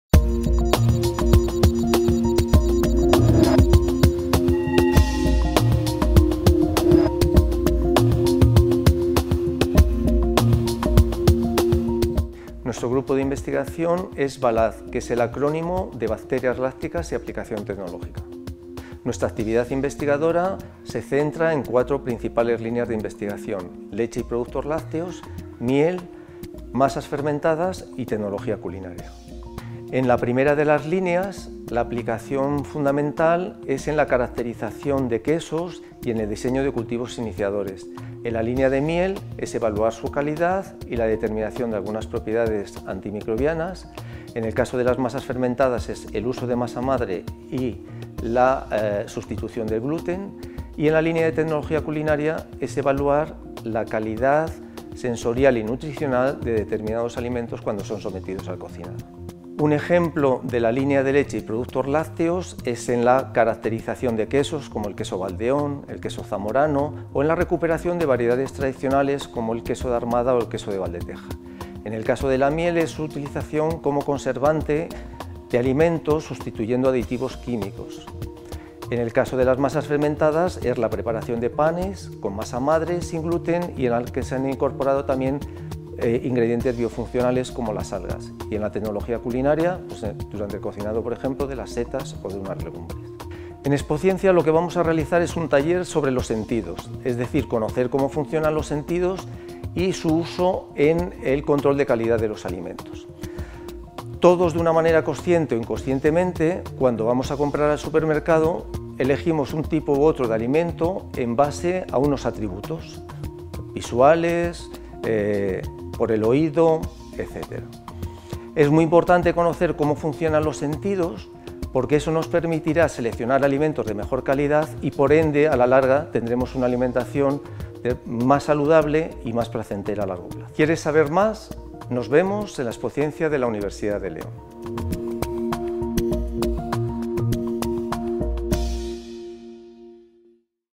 Expociencia Unileon: BALAT (Bacterias Lácticas y Aplicación Tecnológica)
BALAT desarrolla 4 grandes líneas de investigación en torno a: la leche y productos lácteos, la miel y los derivados de la colmena, las masas fermentadas y la tecnología culinaria.